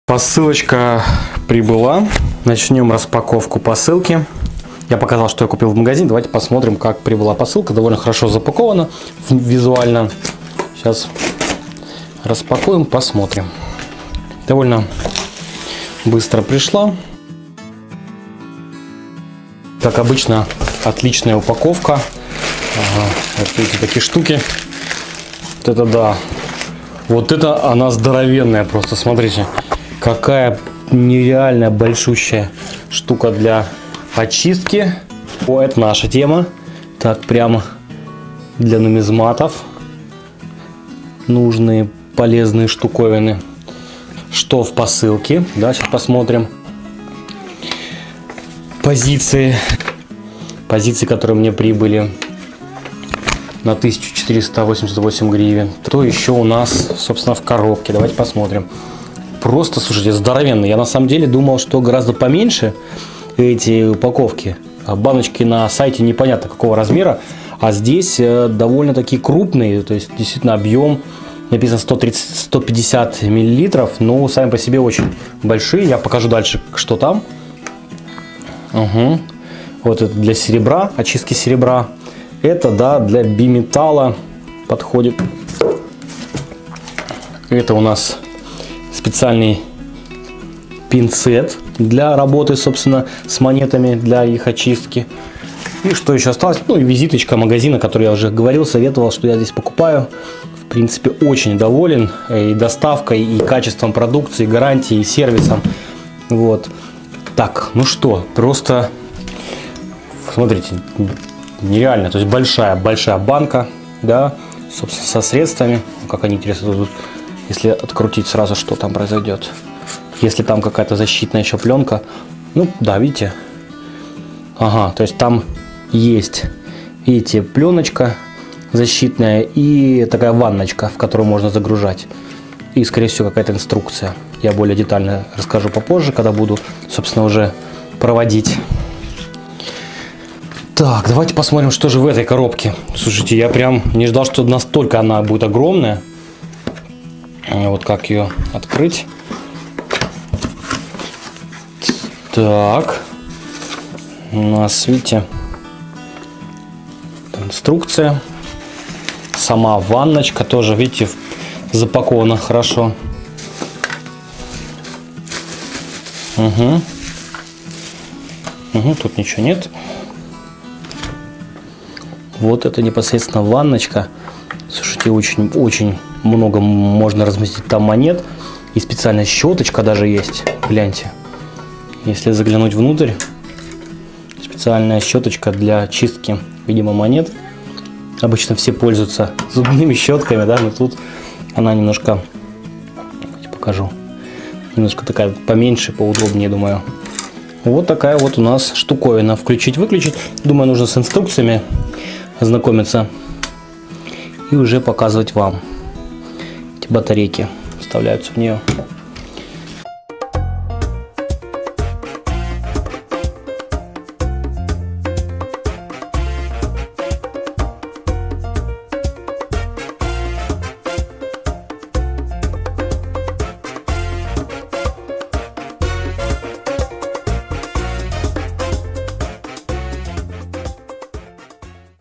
Открываю посылку для нумизмата. Чем почистить монеты. Средства Leuchtturm
Я получил посылку с специальными средствами для чистки монет фирма Leuchtturm
Давно хотел заказать.
Какие монеты почистить и показать вам? Пишите в комментариях.
- Прибор для чистки монет, Leuchtturm
- Средство для очистки серебряных монет и серебра

❤️Подписался на  канал - выиграл монету!